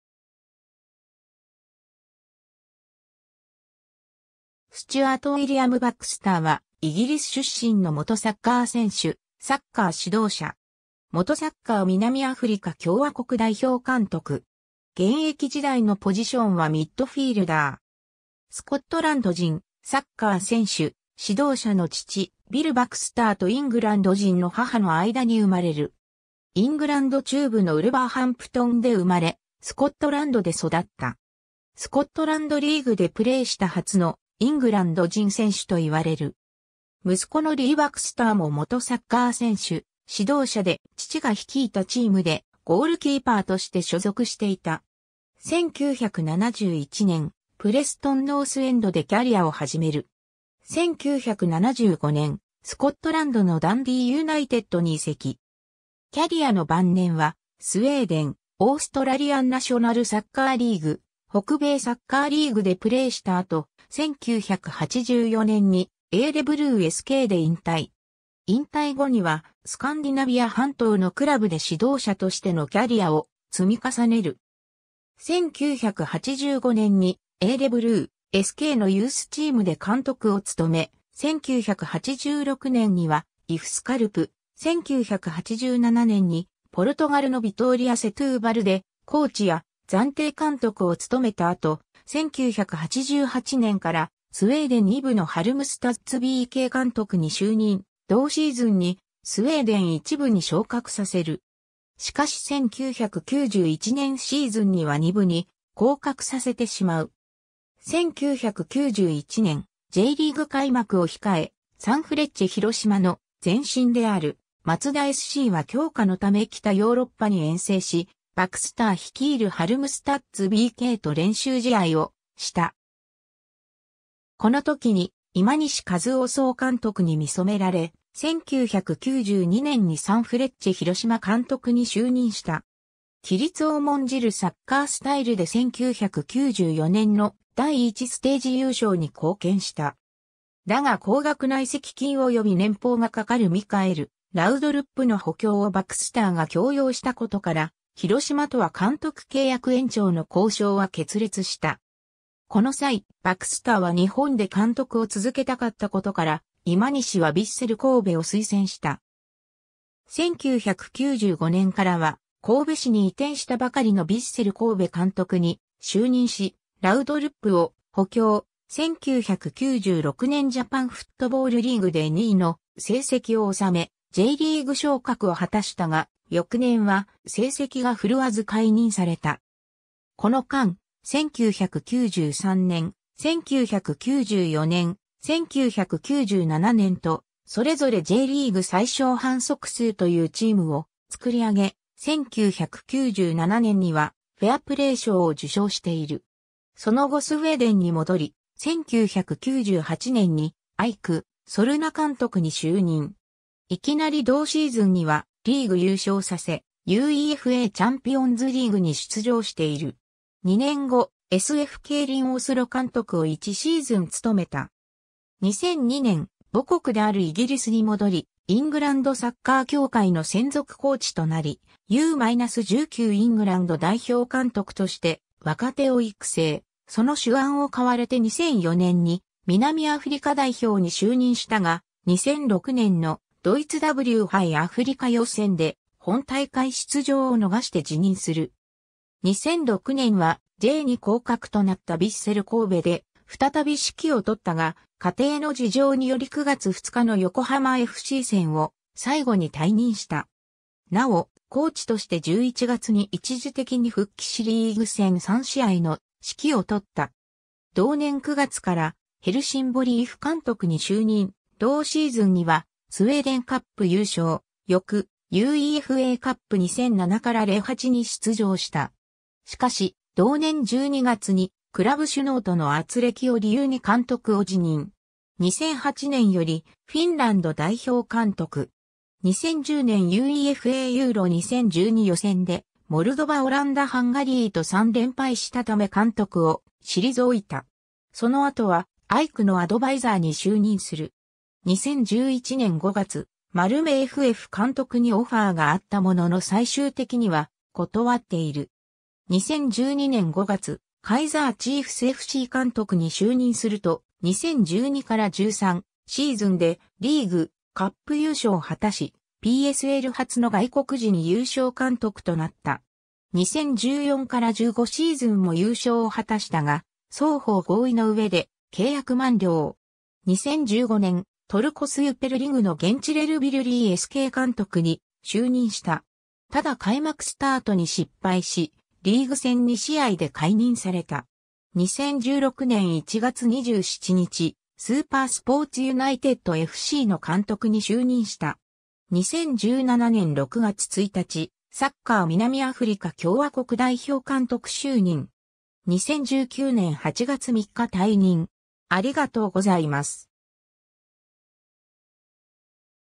スチュアート・バクスター
プレストン・ノースエンドFCの選手
ダンディー・ユナイテッドFCの選手
ストックポート・カウンティFCの選手
サウス・メルボルンFCの選手
スチュアート・ウィリアム・バクスター（Stuart William Baxter 1953年8月16日 - ）は、イギリス出身の元サッカー選手、サッカー指導者。
元サッカー南アフリカ共和国代表監督。
 スコットランド人サッカー選手/指導者の父ビル・バクスターとイングランド人の母の間に生まれる。
イングランド中部のウルヴァーハンプトンで生まれ、スコットランドで育った。
スコットランドリーグでプレイした初のイングランド人選手と言われる。
 息子のリー・バクスターも元サッカー選手、指導者で、父が率いたチームでゴールキーパーとして所属していた。
 1971年、プレストン・ノースエンドでキャリアを始める。
1975年、スコットランドのダンディー・ユナイテッドに移籍。
キャリアの晩年はスウェーデン、オーストラリアン・ナショナルサッカーリーグ、北米サッカーリーグでプレー した後、1984年にエーレブルーSKで引退。
 引退後にはスカンディナヴィア半島のクラブで指導者としてのキャリアを積み重ねる。
1985年にエーレブルーSKのユースチームで監督 を務め、1986年にはIFスカルプ、1987年にポルトガルのヴィトーリア・セトゥーバル でコーチや暫定監督を務めた後、1988年からスウェーデン2部のハルムスタッズBK監督 に就任、同シーズンにスウェーデン1部に昇格させる。
しかし1991年シーズンには2部に降格させてしまう。
 1991年、Jリーグ開幕を控え、サンフレッチェ広島の前身であるマツダSCは強化のため北ヨーロッパに遠征し、バクスター率いるハルムスタッズBKと練習試合をした。
この時に今西和男総監督に見初められ、1992年にサンフレッチェ広島監督に就任した。
規律を重んじるサッカースタイル で1994年の第1ステージ優勝に貢献した。
だが高額な移籍金および年俸がかかるミカエル・ラウドルップの補強をバクスターが強要したことから、広島とは監督契約延長の交渉は決裂した。
この際、バクスターは日本で監督を続けたかったことから今西はヴィッセル神戸を推薦した。
 1995年からは神戸市に移転したばかりのヴィッセル神戸監督 に就任しラウドルップを補強、1996年ジャパンフットボールリーグで2位の成績をおさめ、Jリーグ昇格を果たしたが翌年は成績が振るわず解任された。
 この間、1993年、1994年、1997年とそれぞれJリーグ最小反則数というチームを作り上げ、1997年にはフェアプレー賞を受賞している。
 その後スウェーデンにもどり、1998年にAIKソルナ監督 に就任。
いきなり同シーズンにはリーグ優勝させ、UEFAチャンピオンズリーグに出場している。
2年後、SFKリン・オスロ監督を1シーズン務めた。
 2002年、母国であるイギリスに戻り、イングランドサッカー協会の専属コーチとなり、U-19イングランド代表監督 として若手を育成、その手腕を買われて2004年に南アフリカ代表に就任した が、2006年のドイツW杯アフリカ予選で本大会出場を逃して辞任する。
 2006年はJ2降格となったヴィッセル神戸で再び指揮を執った が、家庭の事情により9月2日の横浜FC戦を最後に退任した。
なお、コーチとして11月に一時的に復帰 しリーグ戦3試合の指揮を執った。
 同年9月から、ヘルシンボリIF監督に就任、同シーズンにはスウェーデンカップ優勝、翌UEFAカップ 2007-08に出場した。
しかし、同年12月にクラブ首脳とのあつれきを理由に監督を辞任。
 2008年よりフィンランド代表監督。
2010年UEFA EURO 2012予選でモルドバ・オランダ・ハンガリーと3連敗したため監督を退いた。
 その後はAIKのアドバイザーに就任する。
2011年5月、マルメFF監督にオファーがあったものの最終的には断っている。
 2012年5月、カイザー・チーフスFC監督に就任すると、2012-13シーズンでリーグ、カップ優勝を果たし、PSL初の外国人優勝監督となった。
2014-15シーズンも優勝を果たしたが、双方合意の上で契約満了。
 2015年、...